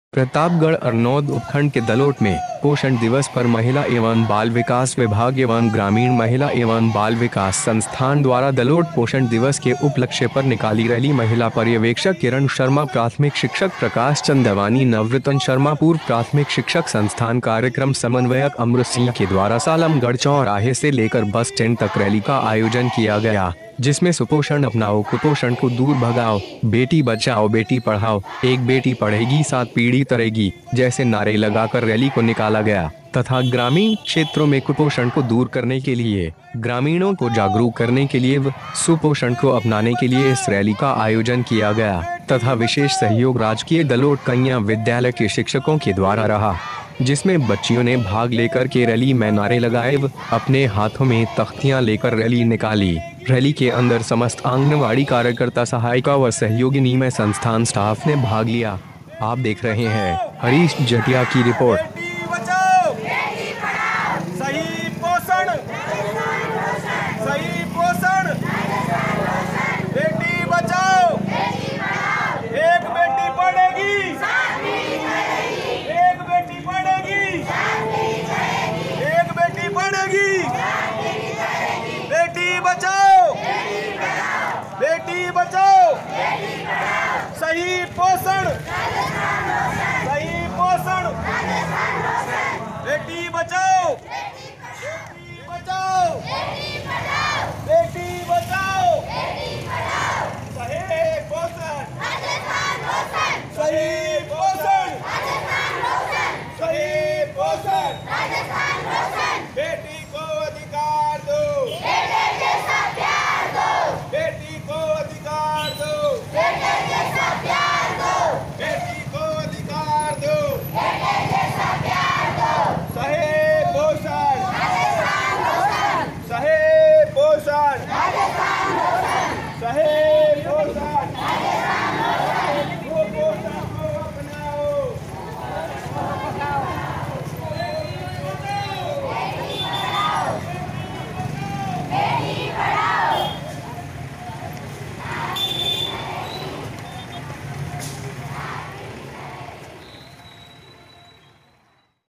दलोट महिला बाल विकास अरनोद व ग्रामीण महिला बाल विकास संस्थान द्वारा निकाली रैली
हरीश जटिया

प्रतापगढ़ अरनोद उपखंड के दलोट में आज पोषण दिवस पर महिला एवं बाल विकास विभाग एवं ग्रामीण महिला एवं बाल विकास संस्थान द्वारा दलोट पोषण दिवस के उपलक्ष पर निकाली रैली महिला पर्यवेक्षक किरण शर्मा प्राथमिक शिक्षक प्रकाश चंदवानी नवरतन शर्मा पूर्व प्राथमिक शिक्षक संस्थान कार्यक्रम समन्वयक अमरसिंह के द्वारा सालम गढ़ चौराहे से से लेकर  बस स्टैंड तक रैली का आयोजन किया गया जिसमें सुपोषण अपनाओ कुपोषण को दूर भगाओ बेटी बचाओ बेटी पढ़ाओ एक बेटी पढ़ेगी सात पीढ़ी तरह की के नारे लगाकर रैली को निकाला गया तथा ग्रामीण क्षेत्रों में कुपोषण  को दूर करने के लिए ग्रामीणों को जागरूक करने के लिए वह सुपोषण को अपनाने के लिए इस रैली का आयोजन किया गया तथा विशेष सहयोग राजकीय दलोट कन्या विद्यालय के शिक्षकों के द्वारा रहा जिसमें बच्चियों ने भाग लेकर के रैली मैं नारे लगाए वह अपने हाथों में तख्तियां लेकर  रैली निकाली रैली के अंदर समस्त आंगनबाड़ी कार्यकर्ता सहायिका वे सहयोगिनी मैं संस्थान स्टाफ ने भाग लिया।